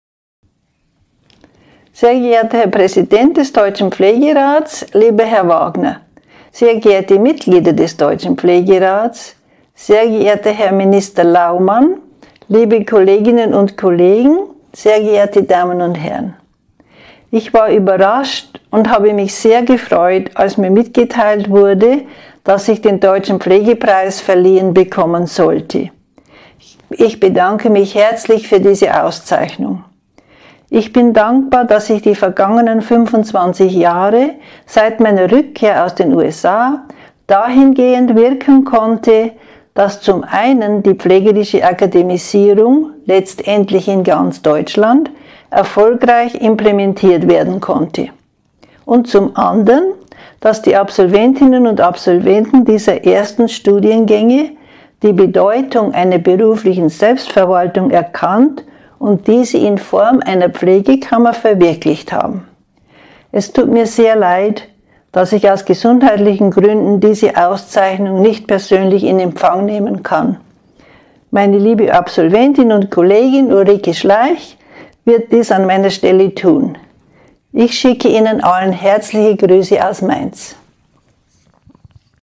Grußbotschaft Edith Kellnhauser
Edith Kellnhauser, ältestes Mitglied der Pflegekammer Rheinland-Pfalz, hat den Ehrenpreis des Deutschen Pflegepreises 2019 erhalten. In ihrer Grußbotschaft bedankt sie sich für die Auszeichnung.